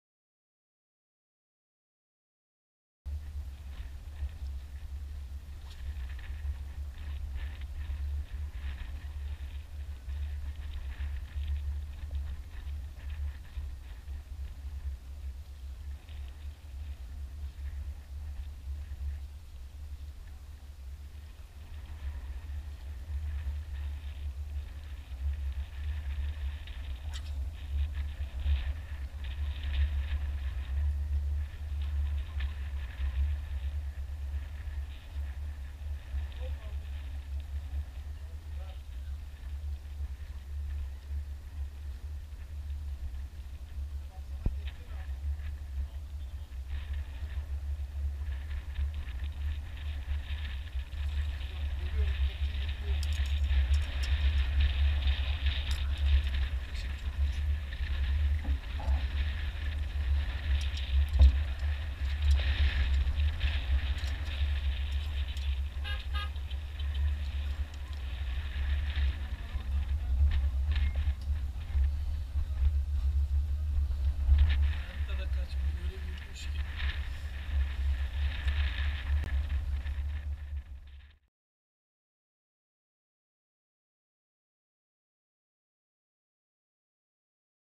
Kaşıkla Lüfer Avı
Meraya varır varmaz oltamı ziyaret eden yakışıklı bir lüfer. Avın devamında maalesef başka vuruş olmadı...